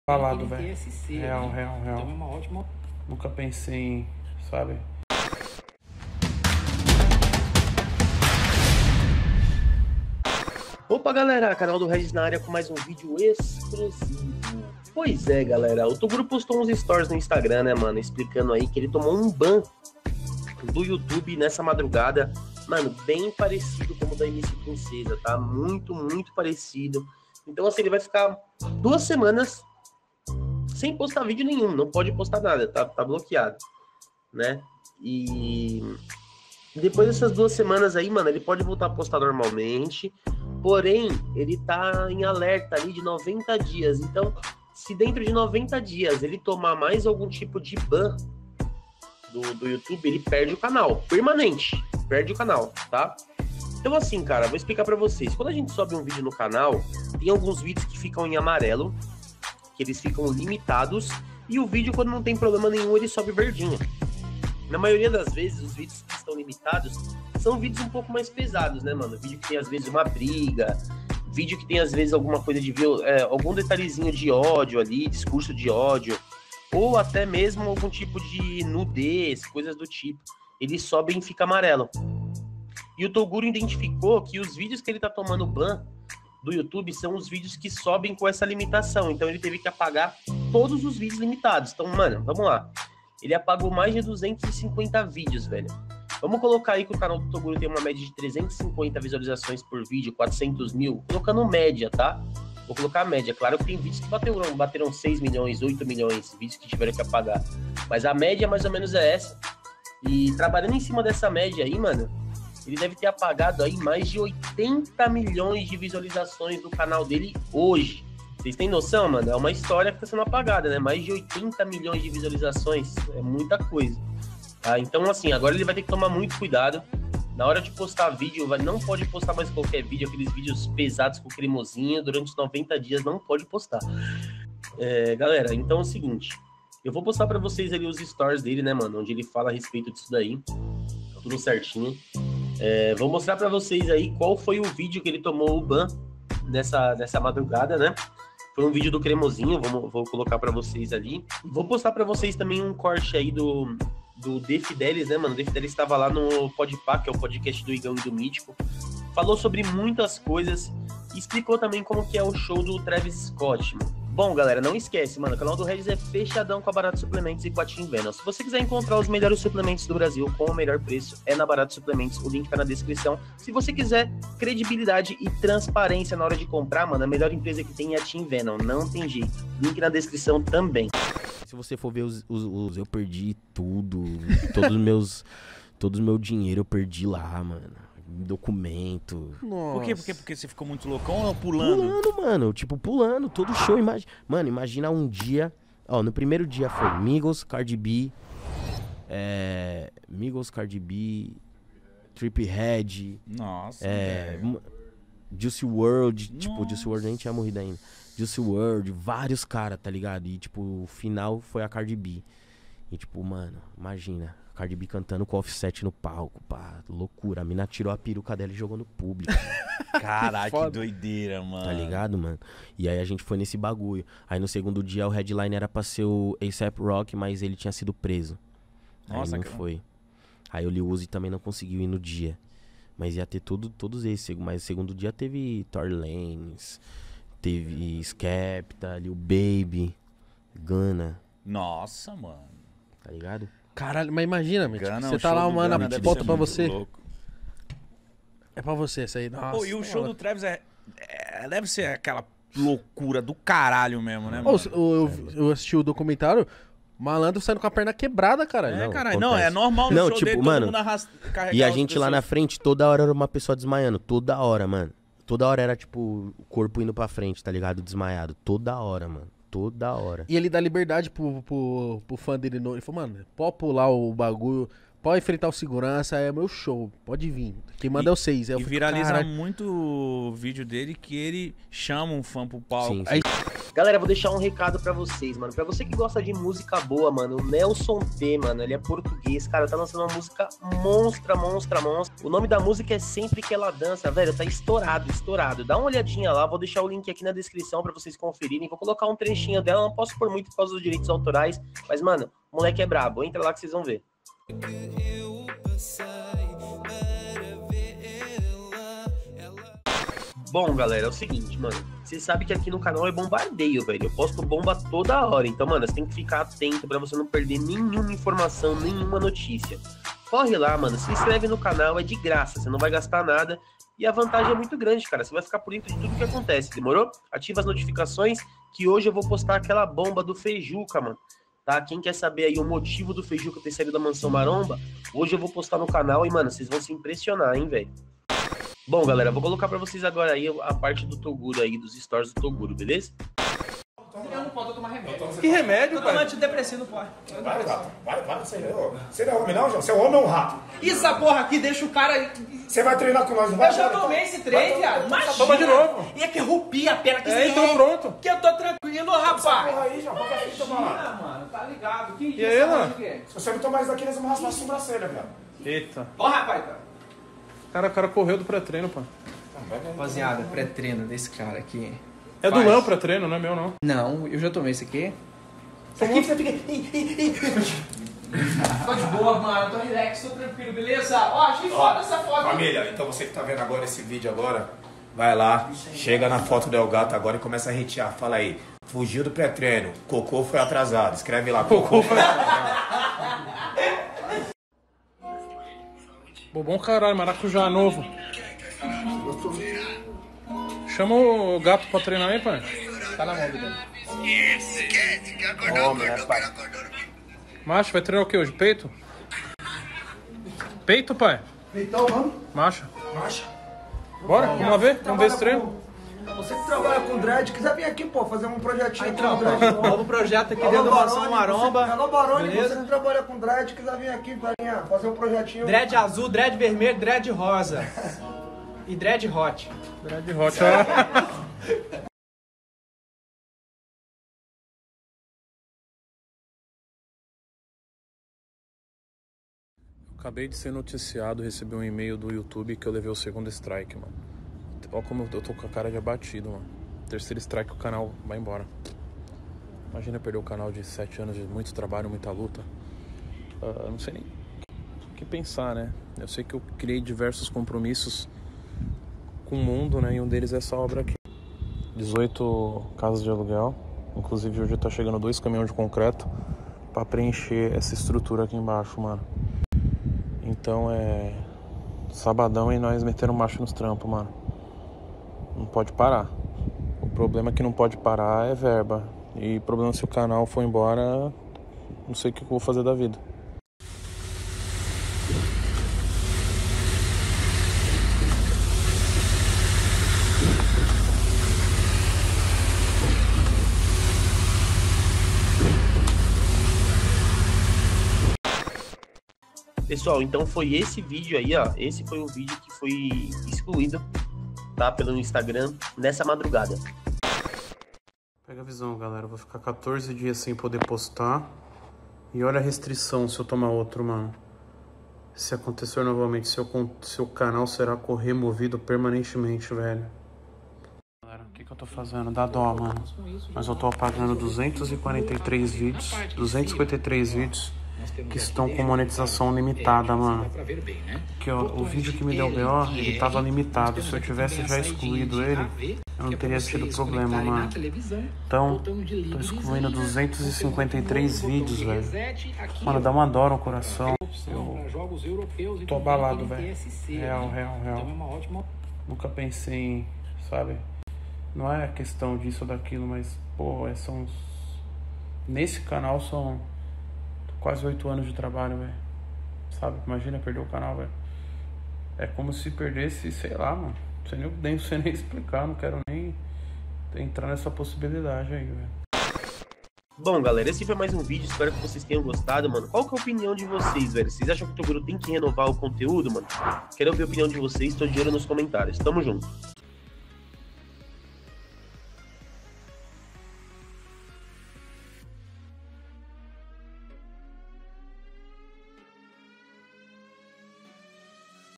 BOMBA! TOGURO APAGA 250 VIDEOS DO CANAL EM BUSCA DO SHAPE INEXPLICÁVEL E PERDE 80 MILHÕES DE VIEWS!
🔥Você encontra suplementos de todas as marcas com o

Opa galera Canal do Regis na área com mais um vídeo exclusivo
Pois é galera O TOGURO DA MANSÃO MAROMBA TOMOU UM BAN NO EM BUSCA DO SHAPE INEXPLICÁVEL e pode perder o canal!
Ele apagou hoje mais de 250 vídeos do canal e pode perder mais de 80 milhões de visualizações!